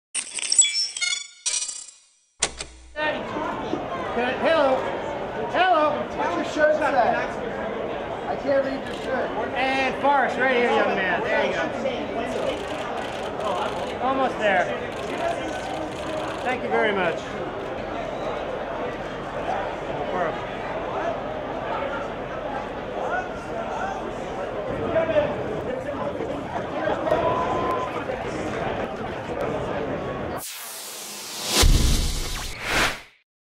Forrest Wheeler “Ant-Man and The Wasp” World Premiere Red Carpet
B-roll footage: Forrest Wheeler on the red carpet at the world premiere of Marvel Studios’ “Ant-Man and The Wasp" held at El Capitan Theater in Los Angeles, California USA on June 25, 2018